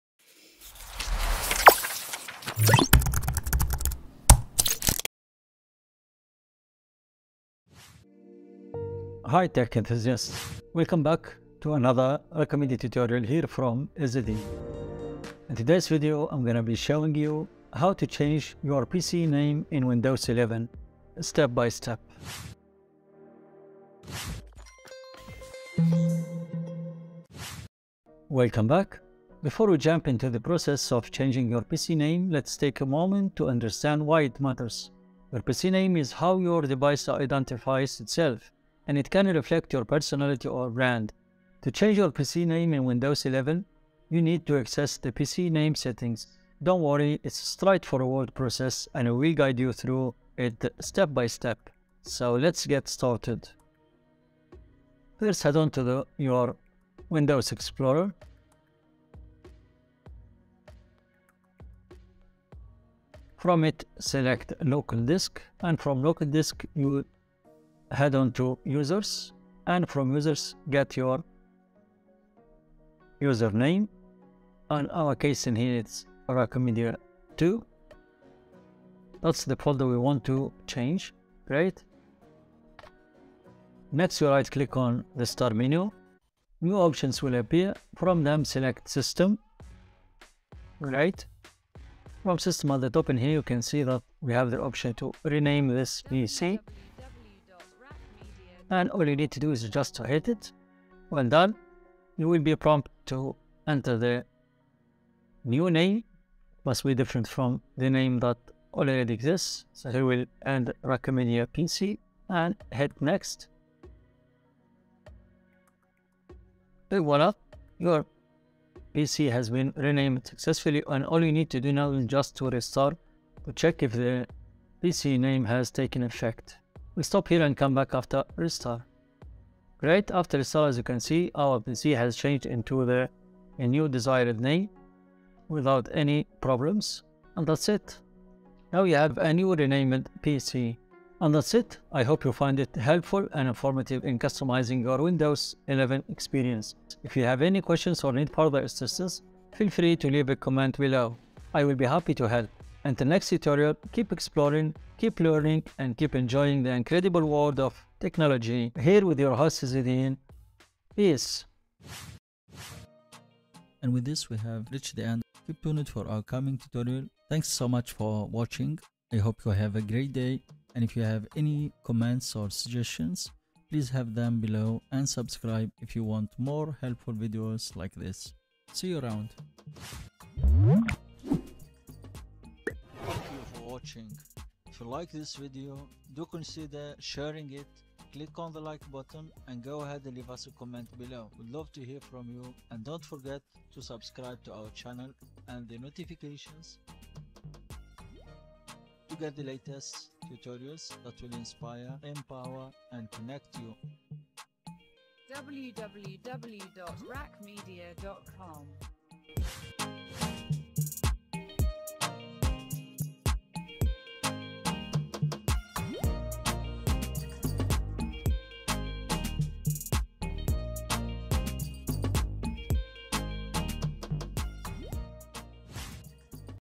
How to Change PC Name Windows 11 ✅ How to Change Administrator Name
Are you ready to take control of your PC and customize it to fit your needs? In this video, we'll show you how to change the name of Windows 11 on your PC as well as how to change the administrator name. This is a great way to make sure that only you have access with full privileges. We'll walk you through step-by-step so it's easy for everyone. Watch our video now and get started!

👍 Please support us & LIKE this Video (Thanks! 💟)